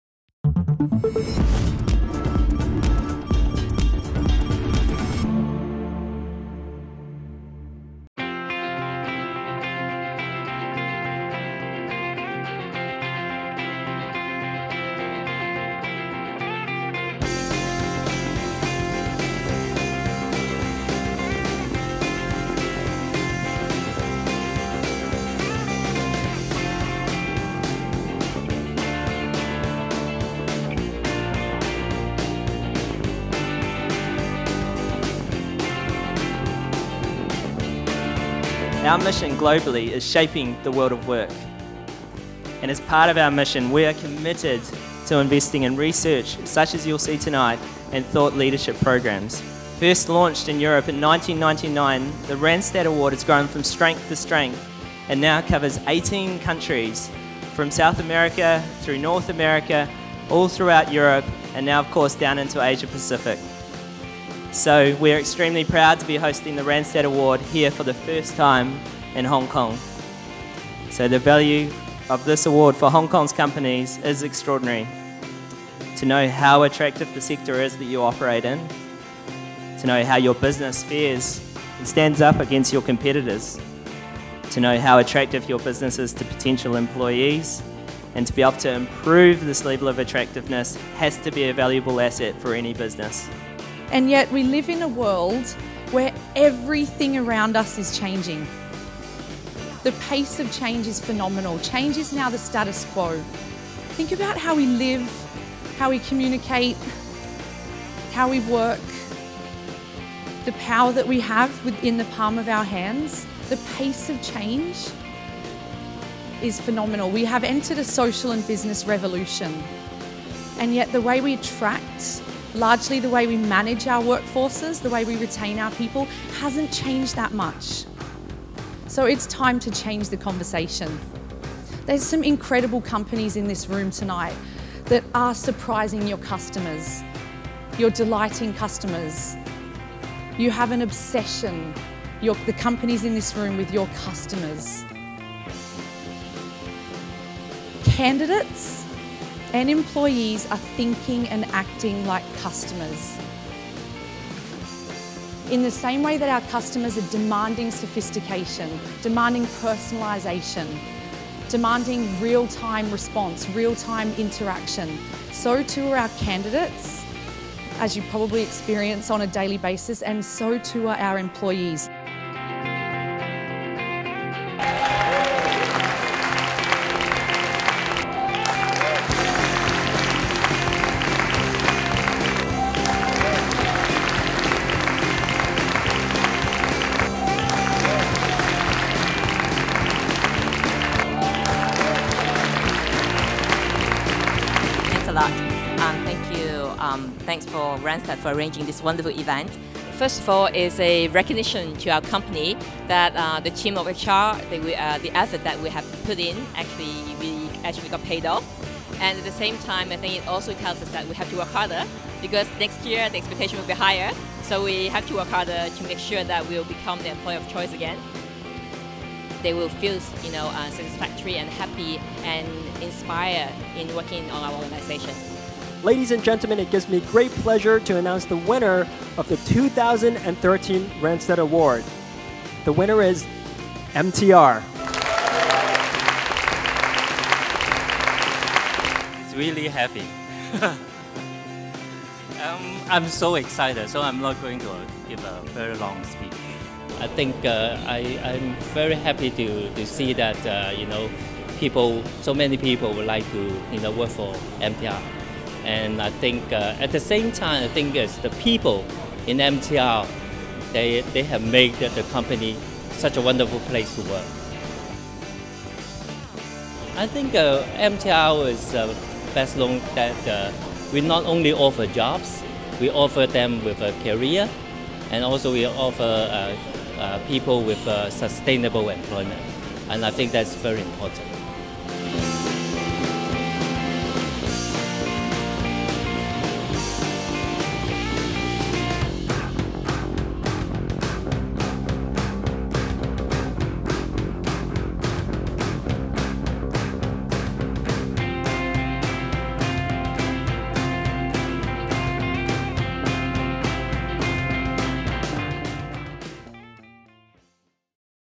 Randstad Award 2013 Gala Dinner Hong Kong
An event to celebrate the most attractive companies in Hong Kong. The Randstad Award is the largest employer branding research programme in the world, spanning across 18 countries.